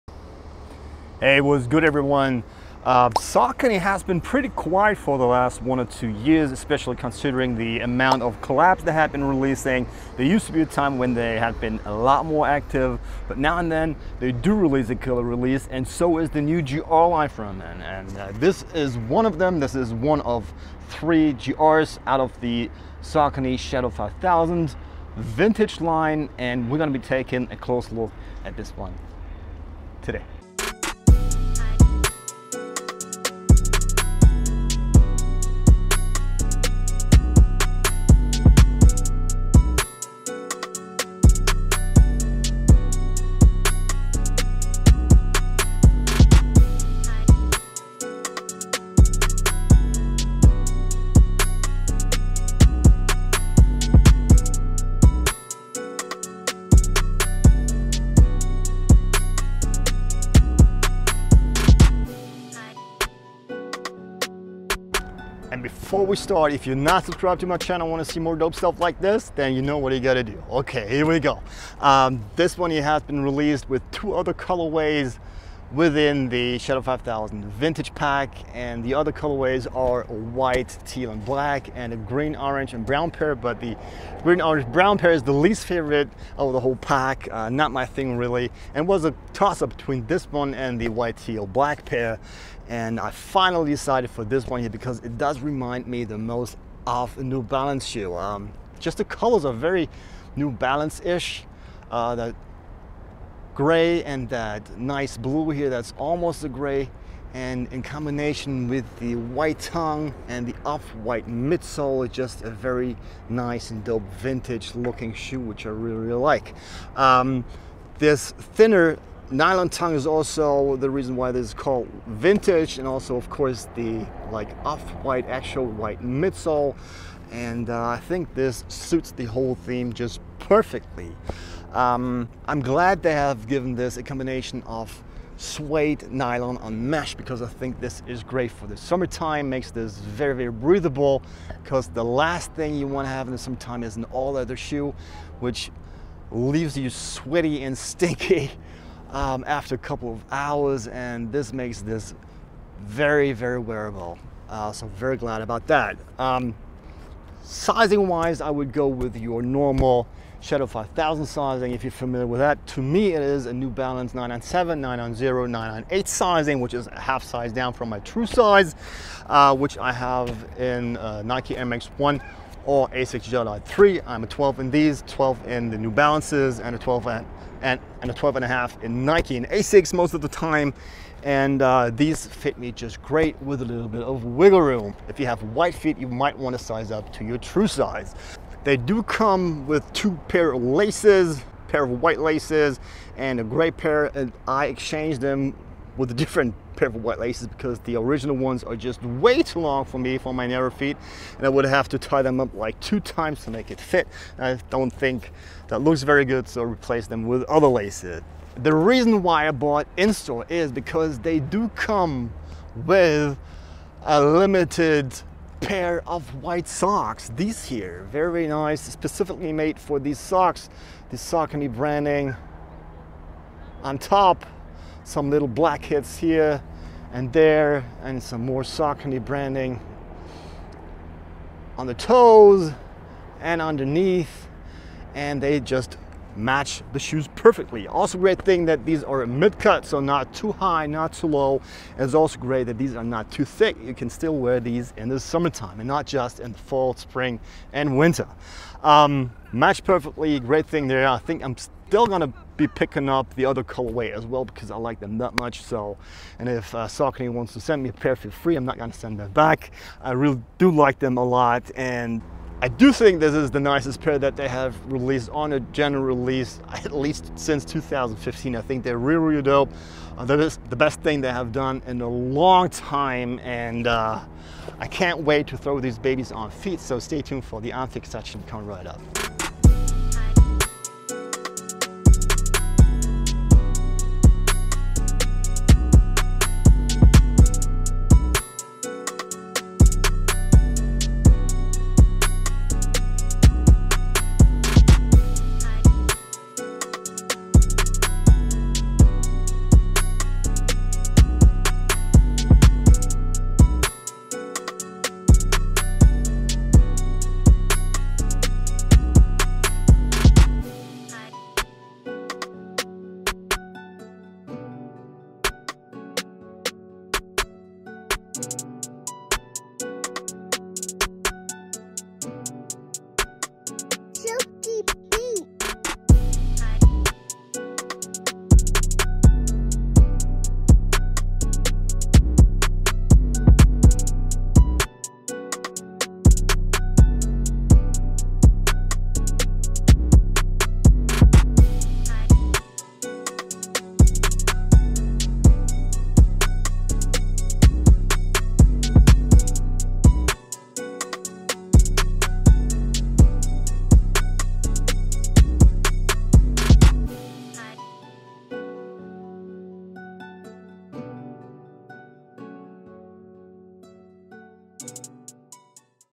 Review & On Feet Video: Best Saucony Shadow 5000 Vintage
Today I'm taking a closer look at the recently released Saucony Shadow 5000 Vintage in the Grey/Blue/White colorway which has released together with two other colorways.

In my opinion this is the best one out of the whole pack, if you want to find out why then click the video!